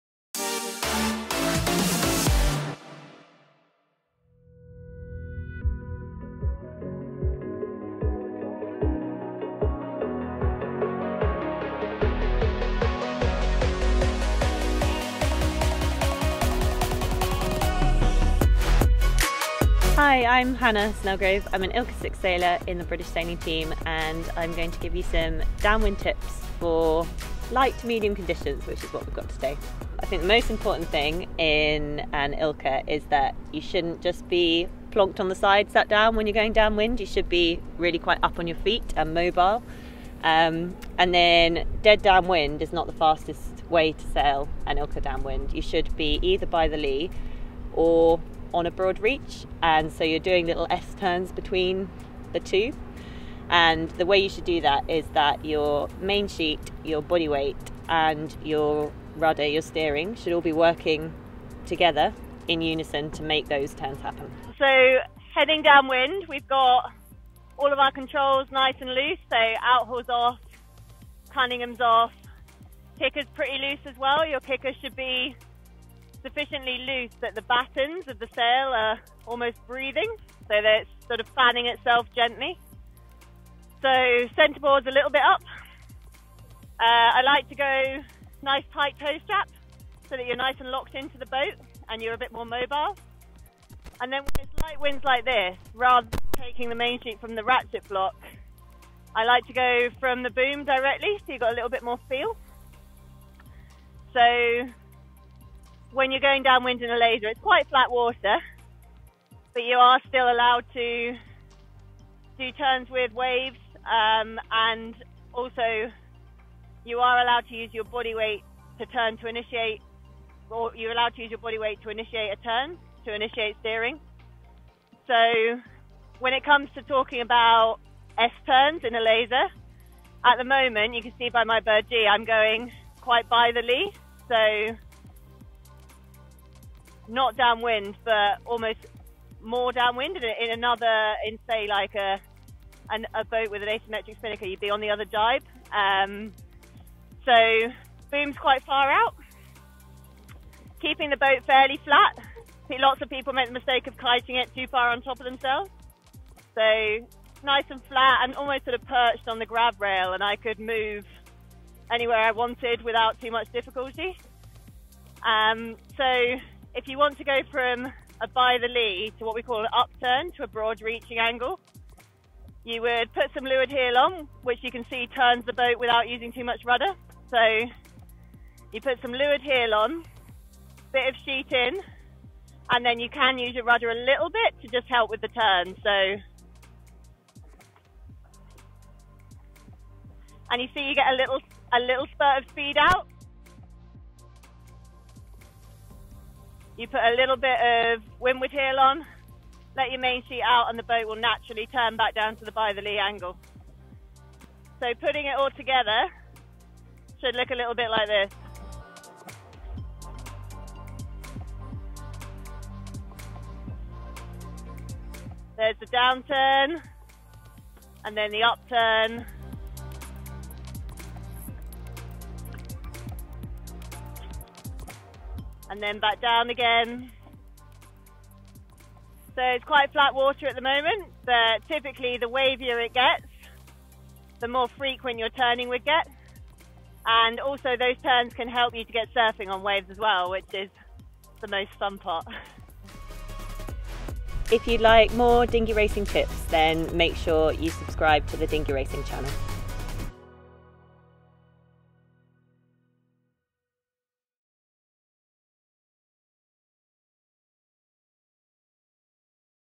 DOWNWIND TECHNIQUE: ILCA (LASER) TECHNIQUE TIPS - How to sail faster with the British Sailing Team
It should be noted that the technique demonstrated by Hannah is only permitted by rule 42 when the waves are big enough to steer through. The technique is shown in this video in flat water so the viewer can see more easily her movements. For more guidance on rule 42 please see the World Sailing interpretations

Hannah Snellgrove from the British Sailing Team gives us some advice on Downwind techniques to use in ILCA (Laser) racing.